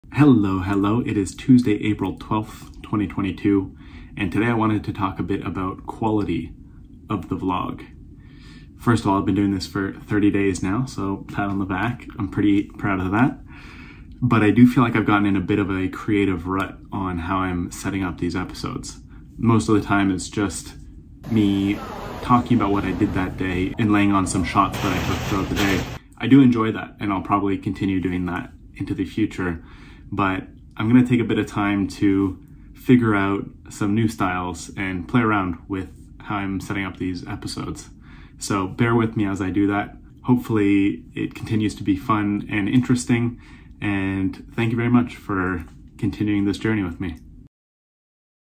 Day 30
Today marks 30 days straight of vlogging! Pretty proud of the consistency so far...now it's time to focus on quality. Not sure if much will change in the short term, but finding some fresh ideas and storytelling styles is top of mind for me these days.

Excited to look back on this video on Day 60 and see the progress!

12APR22 in Singapore